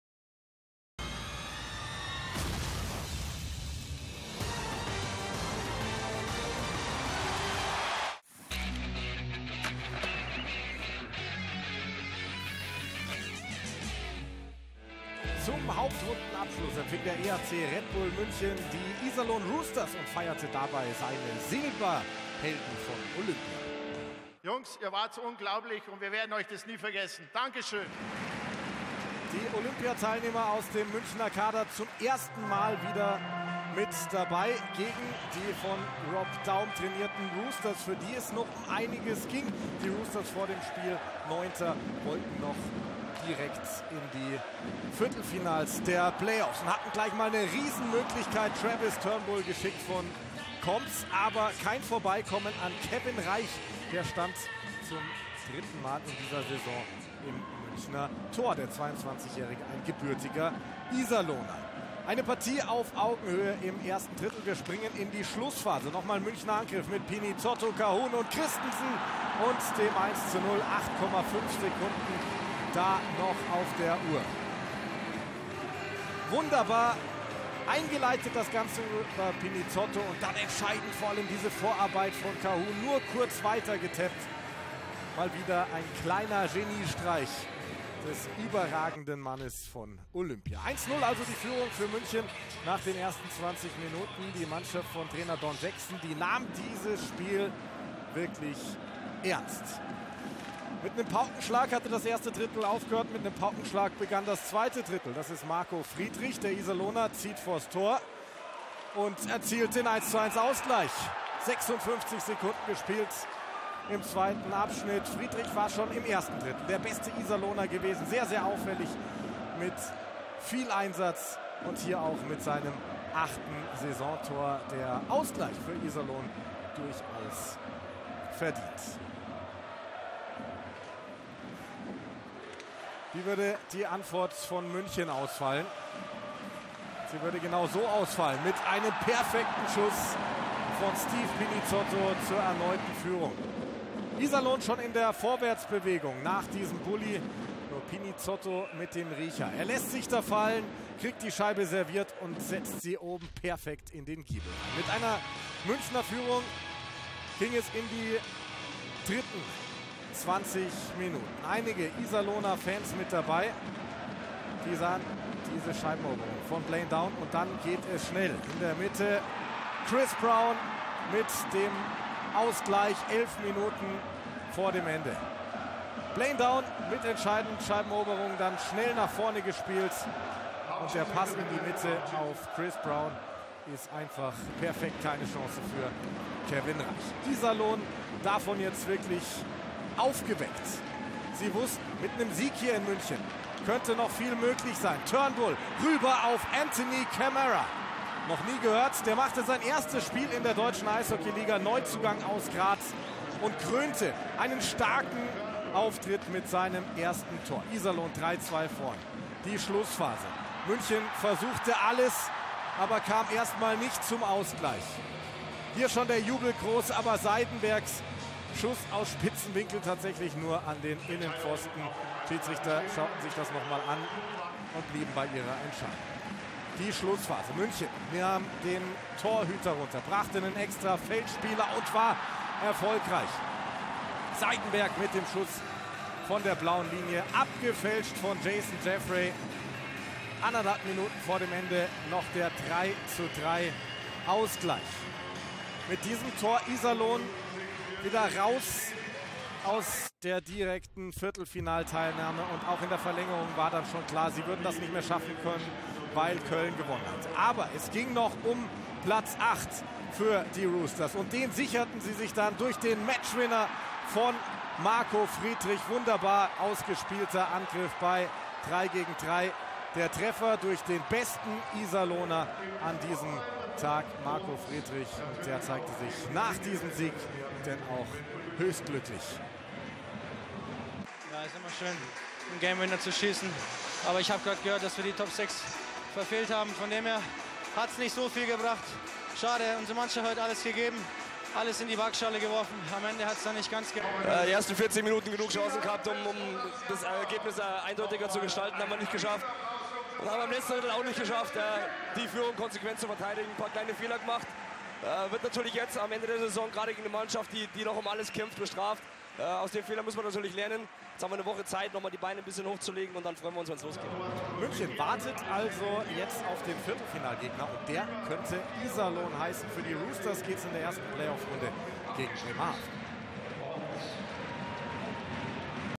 EHC Red Bull München - Iserlohn Roosters | 52. Spieltag, 17/18 | Telekom Sport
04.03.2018, 52. Spieltag der Saison 2017/2018. Die Roosters waren kurz davor, sich mit einem Sieg in München direkt für die Playoffs zu qualifizieren.

Jedoch bekommen sie den Erfolg nicht über die Zeit und müssen die Entscheidung in der Overtime suchen. Für München war der Ausgang der heutigen Partie eher Nebensache. Sie sind als amtierender Meister Sieger der Hauptrunde geworden und zählen erneut als Topfavorit auf die Meisterkrone.

Aufstellungen:

EHC Red Bull München:
Patrick Hager, Michael Wolf, Mads Christensen, Derek Joslin, Ryan Button, Kevin Reich

Iserlohn Roosters:
Blaine Down, Anthony Camara, Christopher Brown, Oskar Eklund, Johan Larsson, Sebastian Dahm